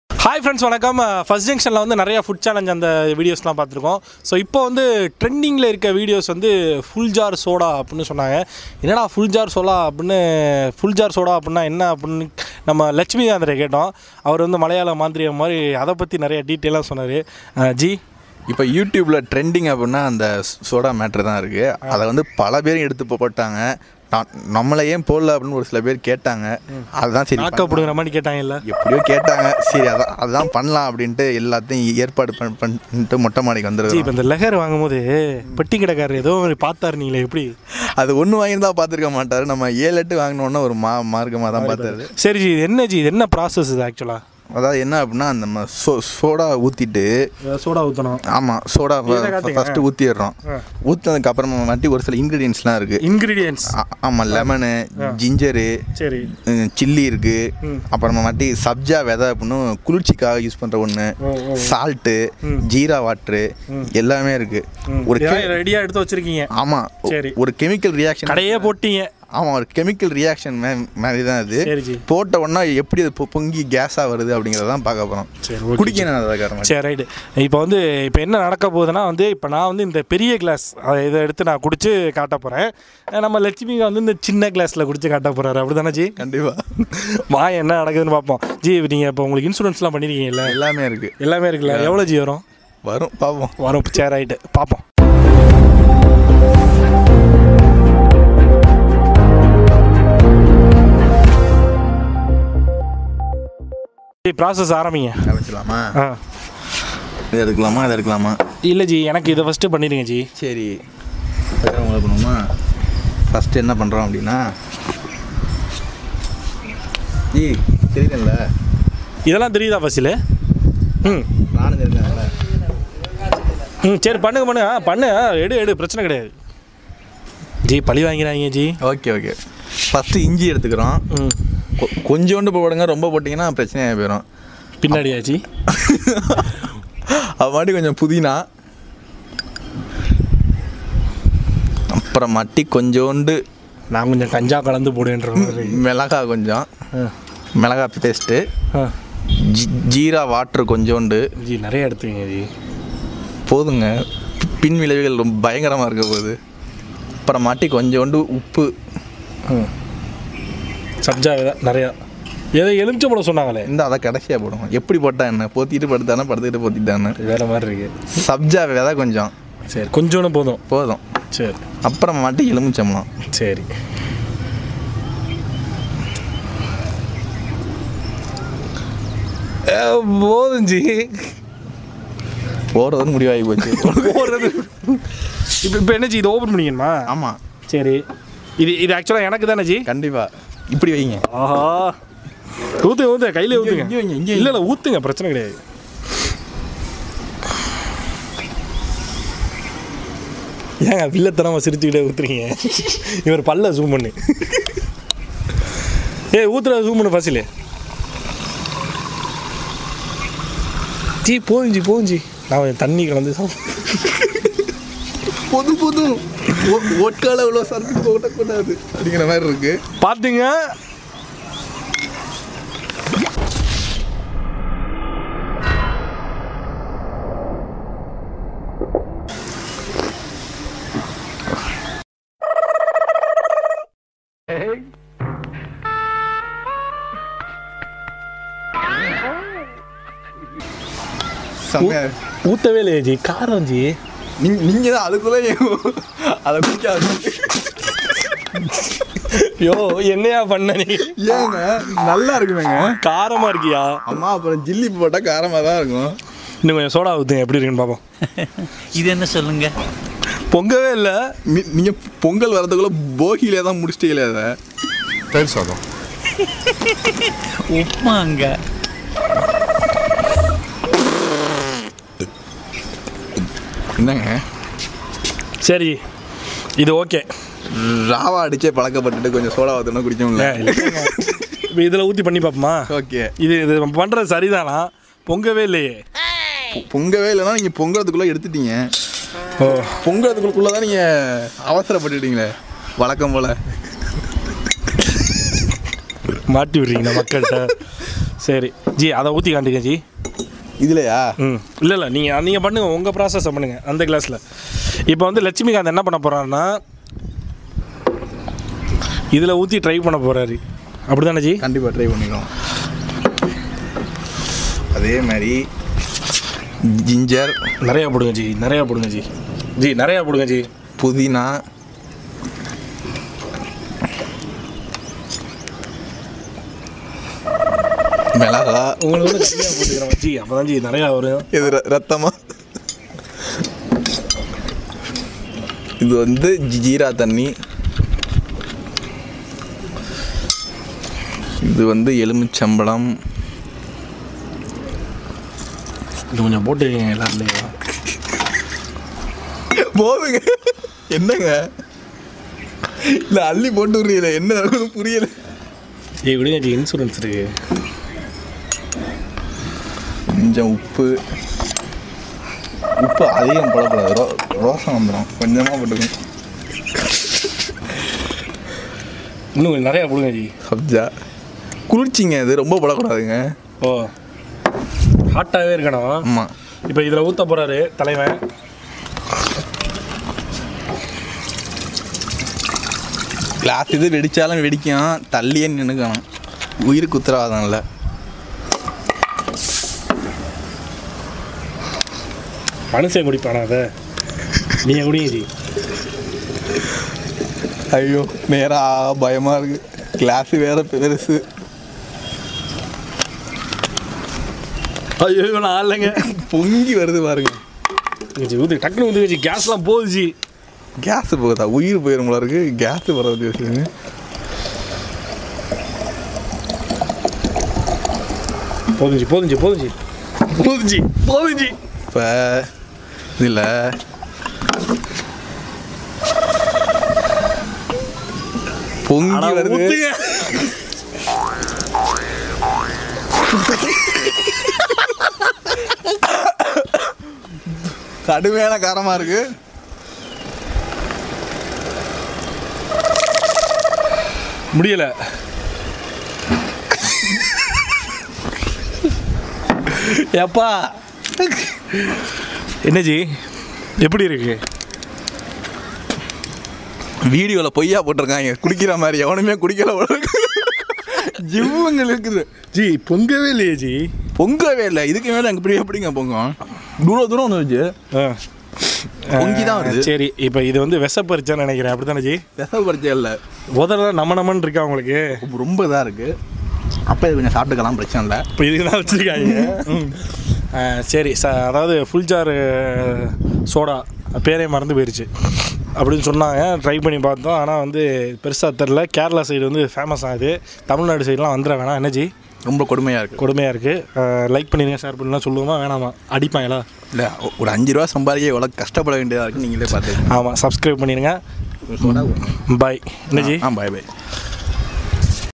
Making FULJAR SODA goes very funny in Tamil | kerala style full jar soda drink recipe in tamil
Fuljar soda drink goes funny wrong | kerala style fuljar soda drink recipe
fuljar soda tamil